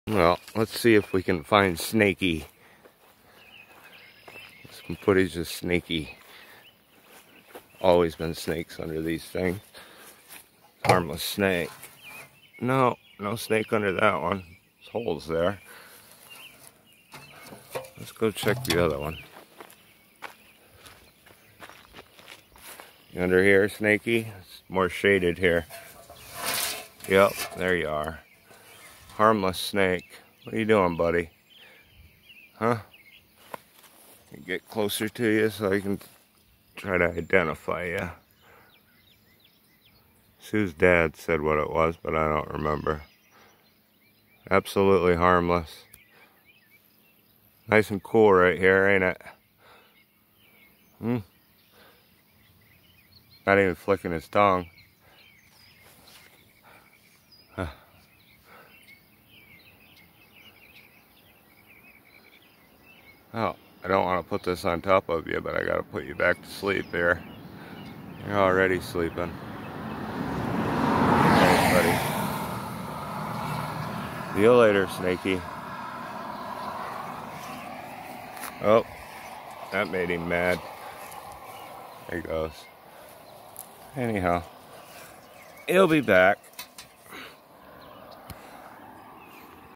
Country Folks Will Survive IN REAL LIFE - Meet Snakey
I was told this was a Garter Snake, but im not 100% sure. Thanks! Stay Safe & Stay Positive!

SNAKES ARE REPTILES. Snakes belong to the animal class reptiles. Reptiles are cold-blooded animals that raise their body temperature by lying in the sun or lower it by crawling into the shade. Their body temperature changes to the temperature of its surroundings.